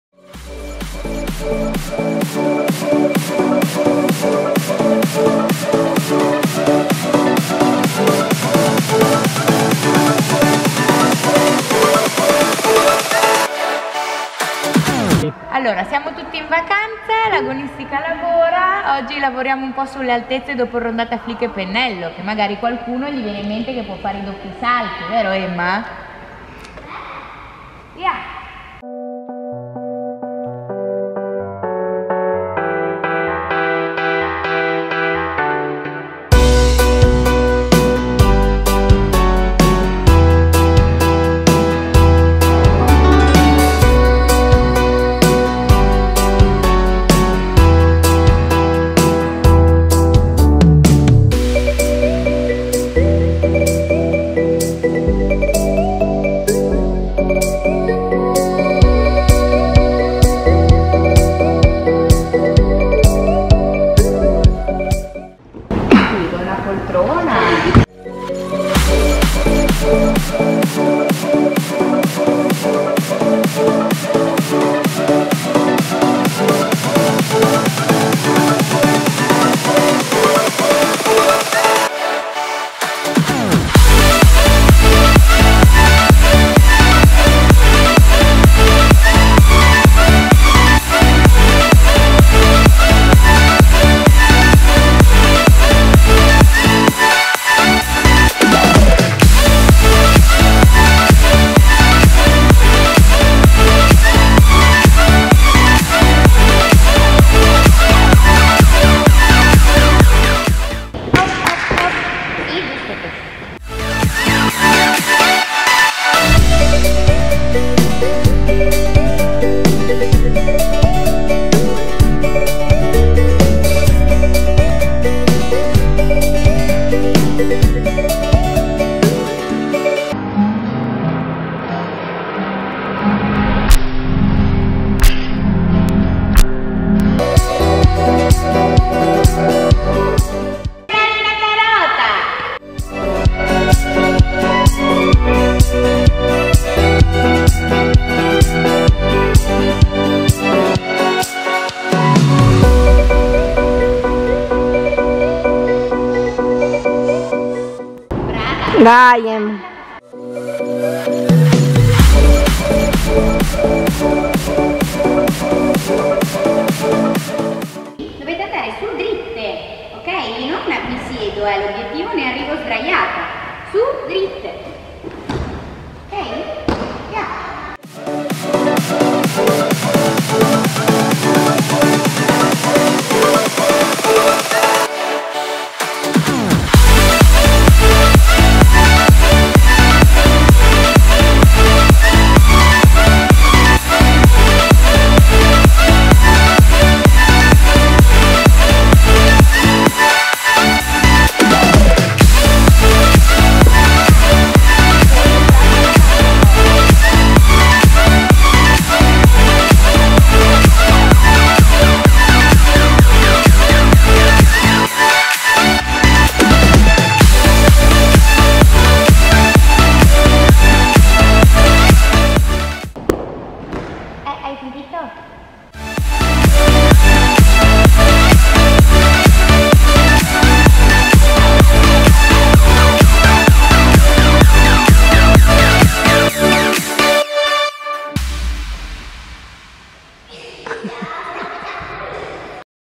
Agonistica piccole - Preparazione ai doppi salti
Bastano il tumbling e un muro di materassoni per prepararci al meglio alle nuove sfide...!

Ti sei già iscritto al canale?
Supportaci con un bel pollicione in su!!!

Grazie per essere passato da qui!